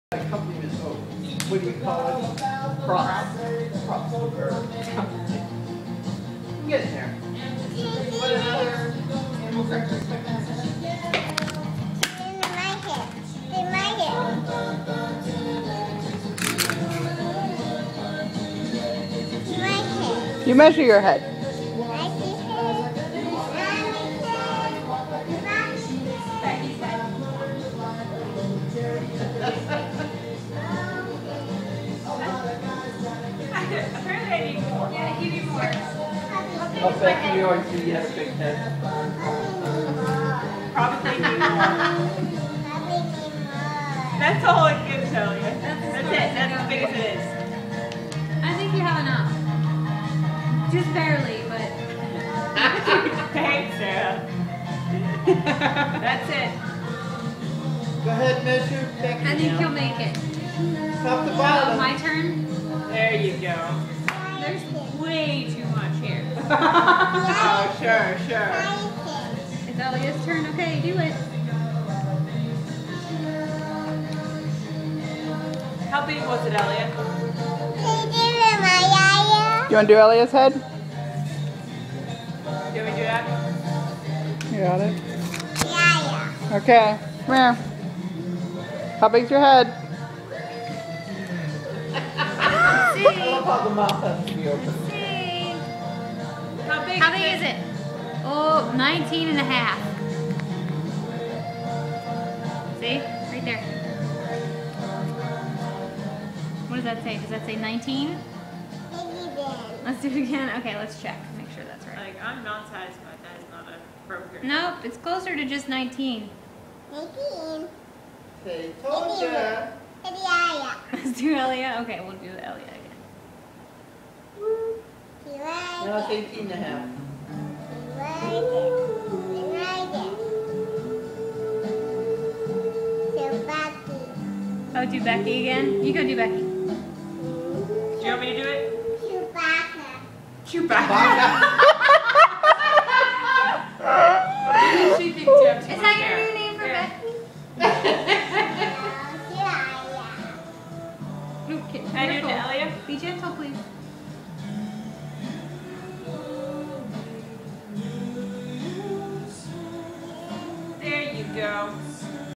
Head Measuring With Ellia
Ellia decided she needed to see how large everyone's heads were!